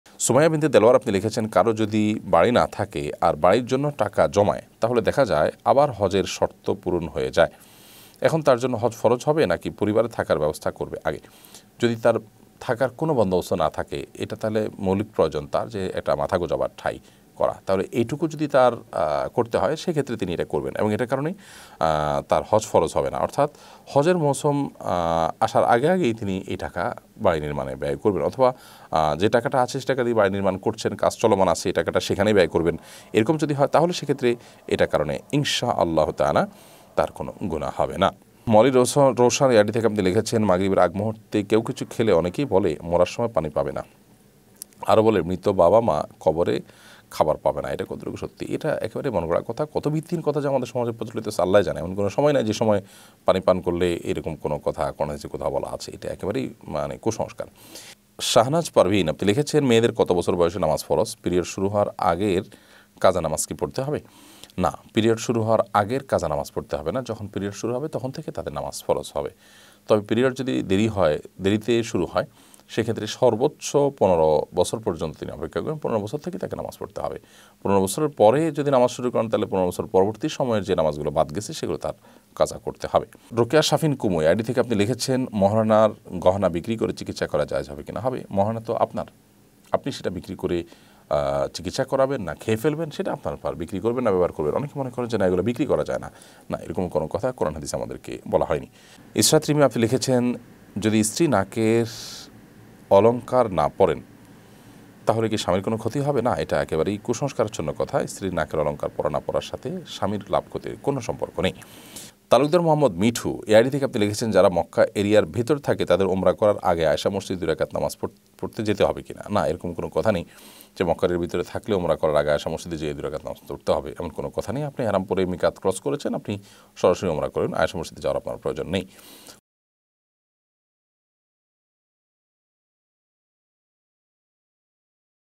বাড়ি করার জন্য জমানো টাকায় কি হজ ফরজ হবে?
বাড়ি করার জন্য জমানো টাকায় কি হজ ফরজ হবে? new waz II waj II Sheikh ahmadullah